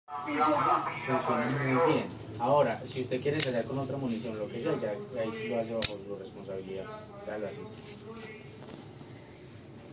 Navaja Automática Salida Frontal gravado lobo
Para nosotros es un gusto entregar a nuestros clientes productos de excelente calidad, para esta ocasión te ofrecemos, la navaja automática de salida frontal color negro. Cuenta con rompe vidrios y gancho sujetador para reata o cinturón  multipropósito.